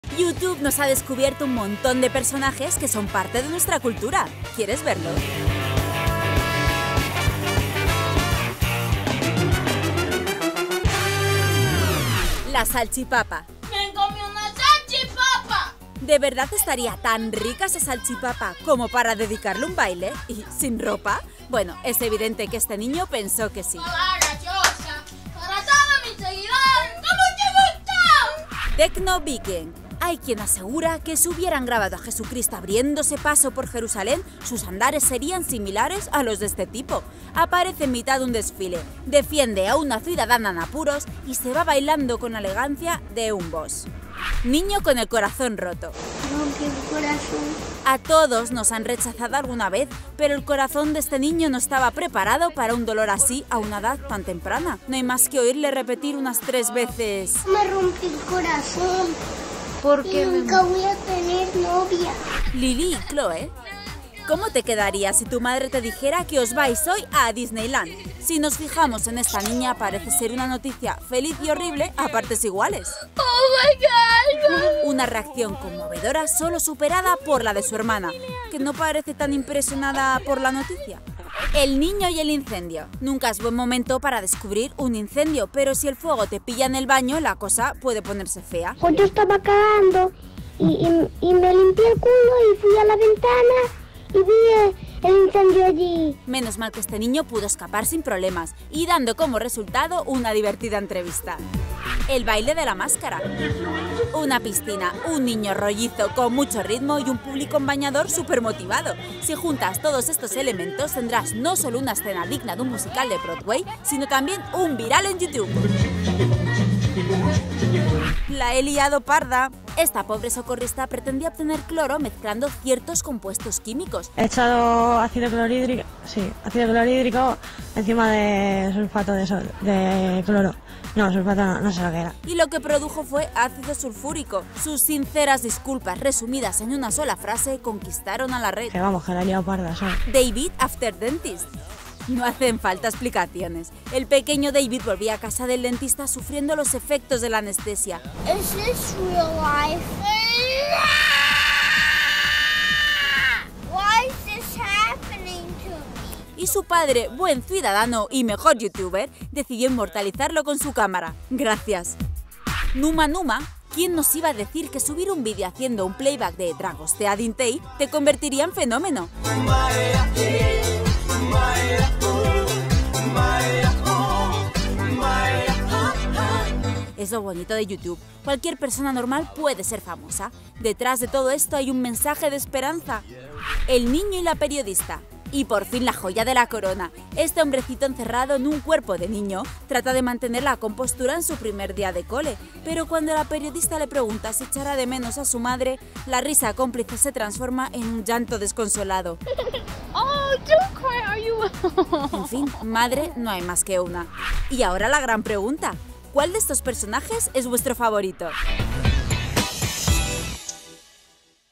10 personajes famosos descubiertos en Youtube
Youtube es una plataforma en la que hemos descubierto muchos talentos. Te traemos 10 personajes desconocidos que publicaron sus vídeos y se hicieron famosos con estos virales. ¿Los habías visto todos?